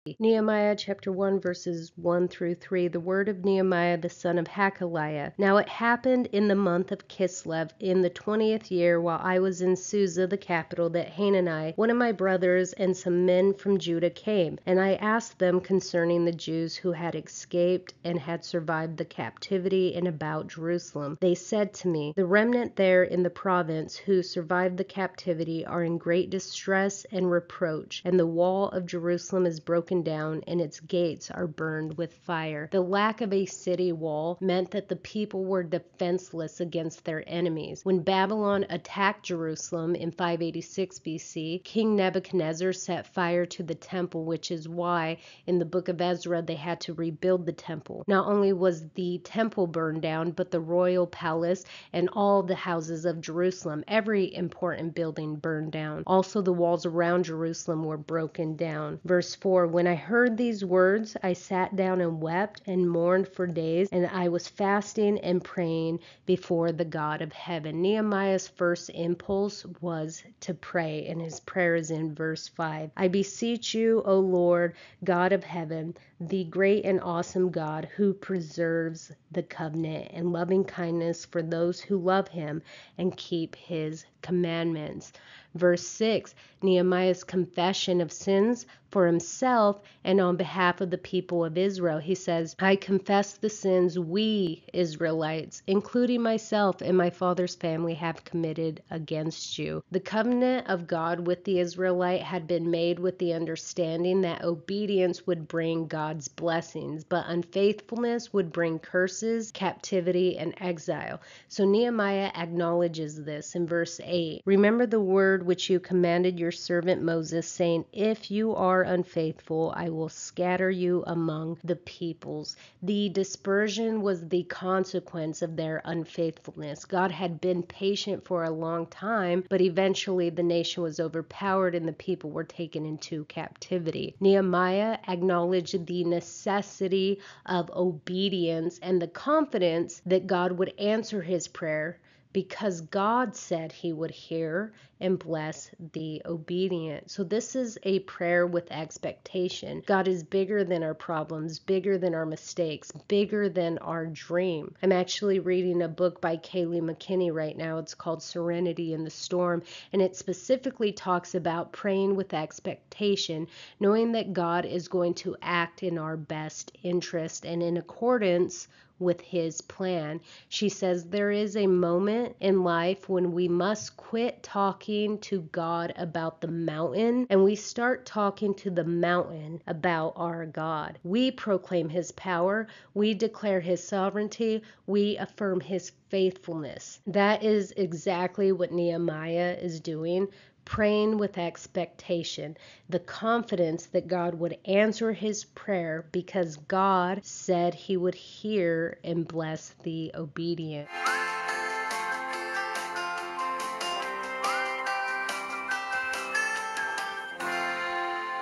Praying with Expectation
Nehemiah's first impulse was to pray. We learn a lesson on praying and obeying with expectation from Nehemiah chapter 1.